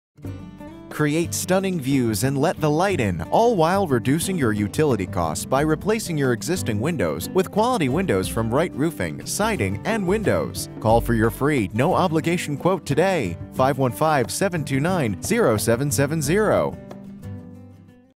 Right Roofing, Siding and Windows -- Des Moines, IA
For over 15 years, Right Roofing, Siding and Windows has been protecting and beautifying hundreds of homes all over Central Iowa. We use top quality materials and professional crews to make sure each job is done right so that it will give you years of worry-free enjoyment, security and protection from the elements. Ryan Johnson, Owner, personally visits every project to ensure they are completed to the owner's 100% satisfaction.